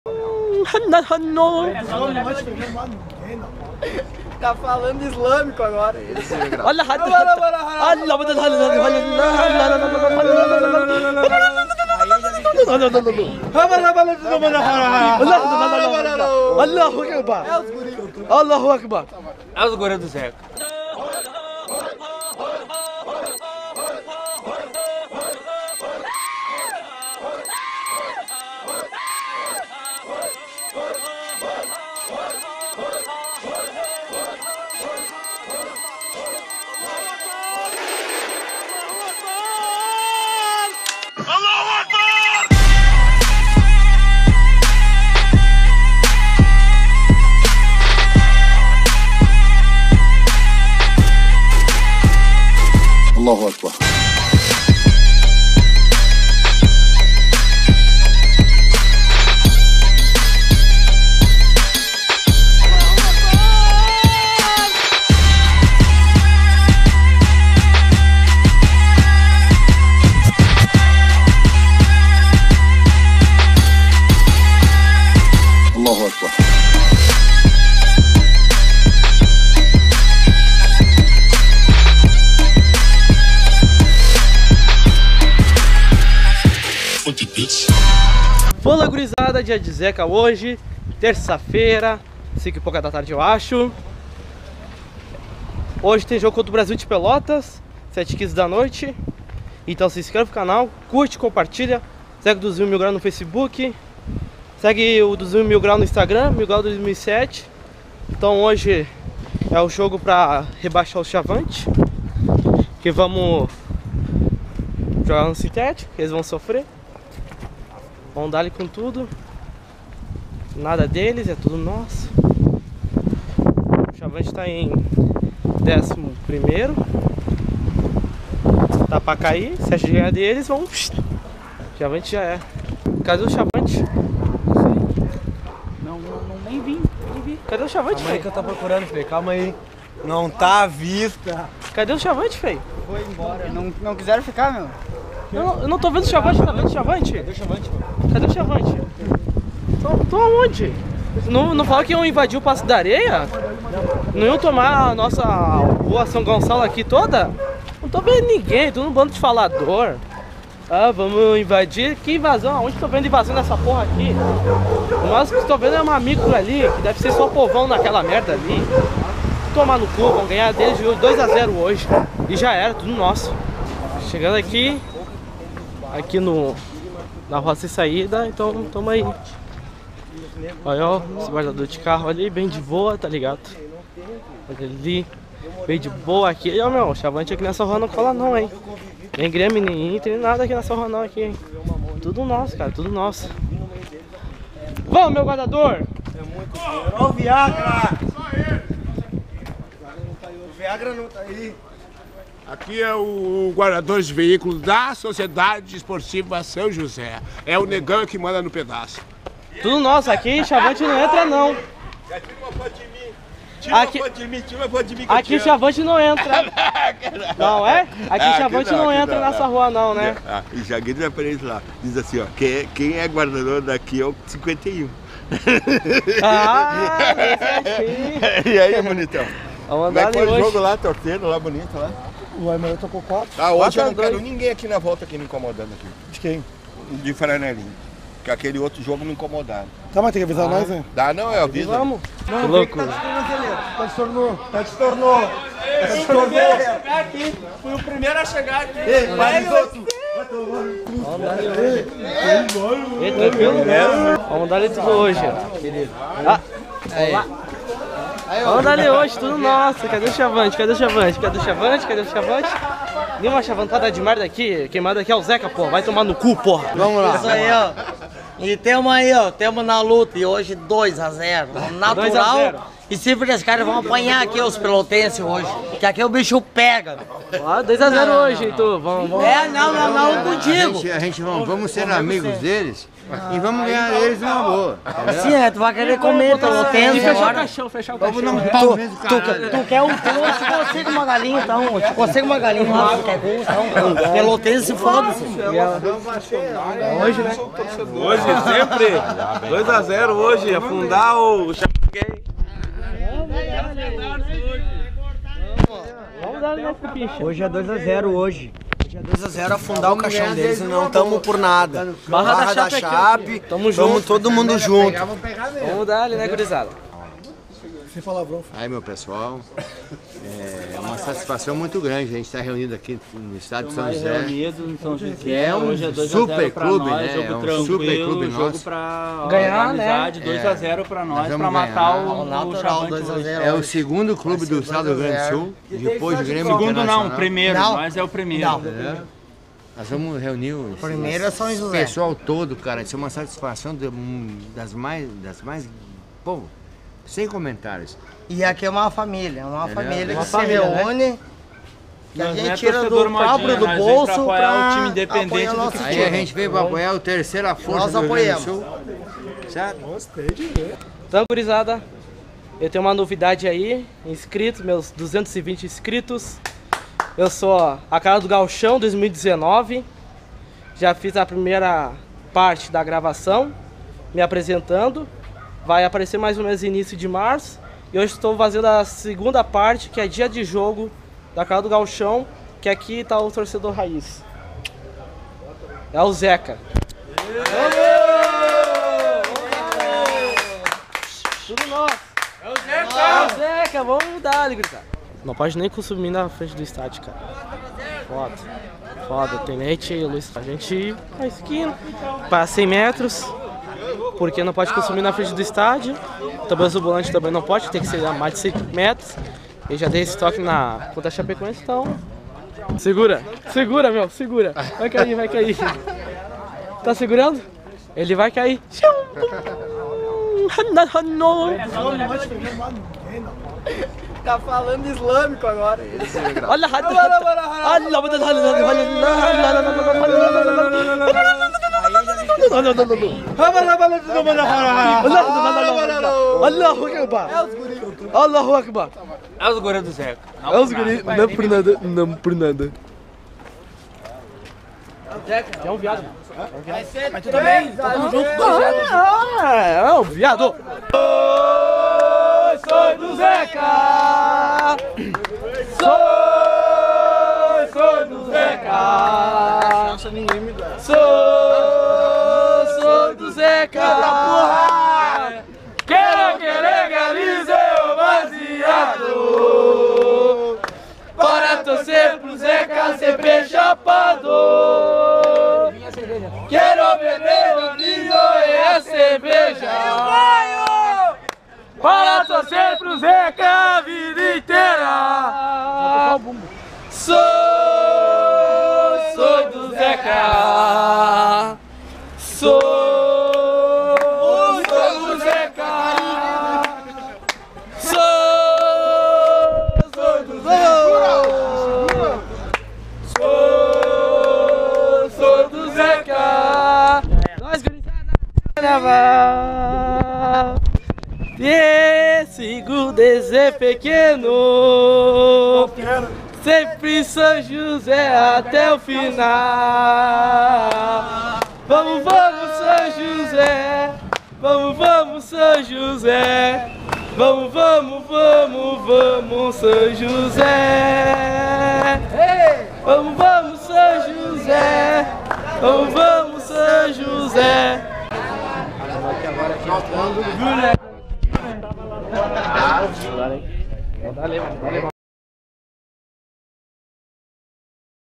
AQUELE ESQUENTA DE LEVEZIN NOS JOGOS ( 1ªPARTE )
SÃO JOSÉ 1 X 3 Brasil de Pelotas - Campeonato Gaúcho 2019 - 8ª Rodada - Estádio Passo D´Areia - Porto Alegre-RS.

DIA DE ZECA #29

 Então galera, a primeira parte como de costume a galera reunida, tomando trago e tal, mas com uma novidade que falei no vídeo!